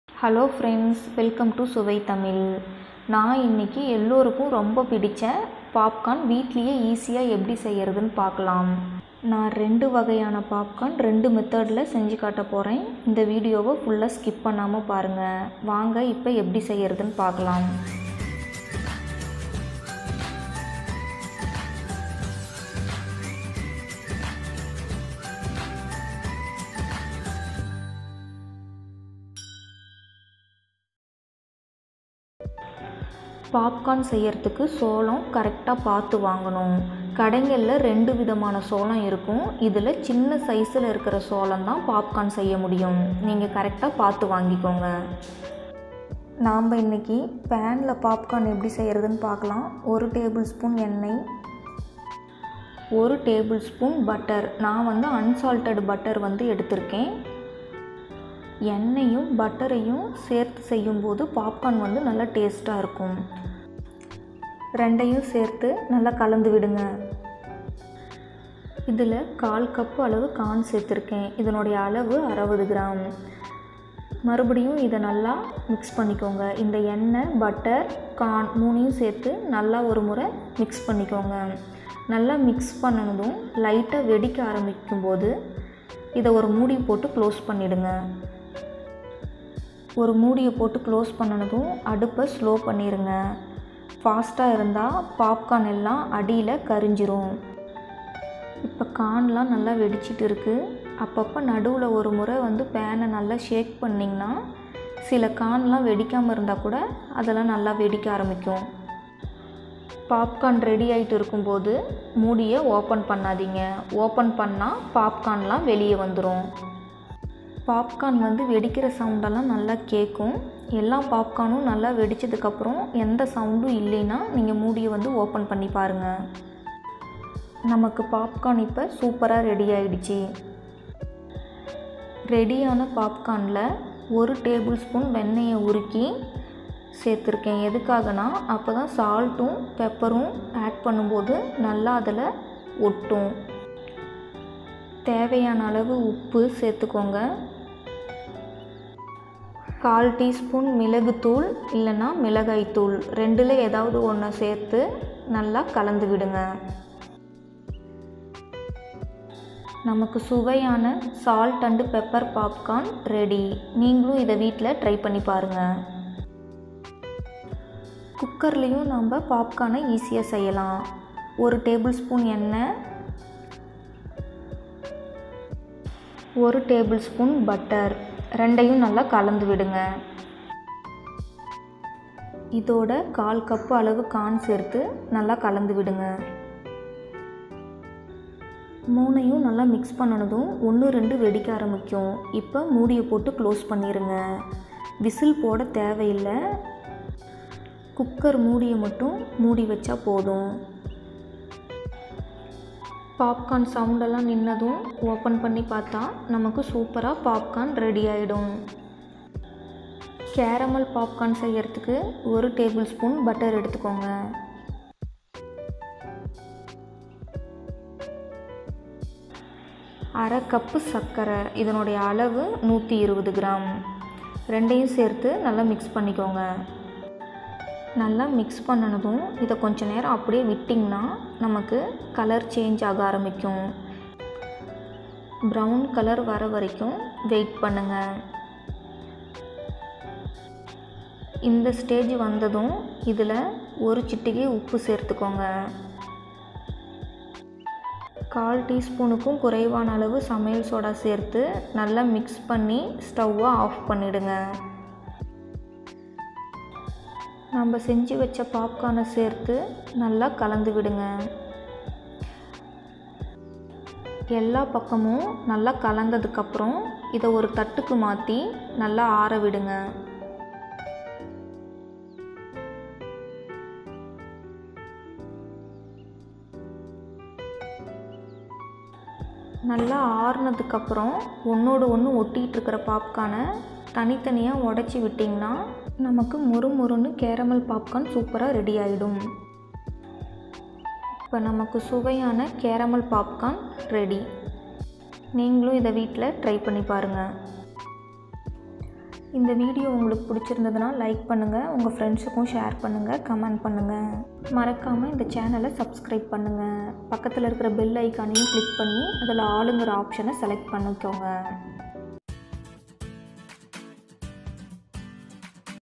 popcorn recipe in Tamil | salt & pepper popcorn recipe | caramel popcorn recipe | snacks recipe
Ingredients:

 Salt & pepper popcorn:
   Dry corn - 1/4 cup (60g)
   Butter - 2tbsp

 Caramel popcorn:
   Butter - 2tbsp
   oil - 1tbsp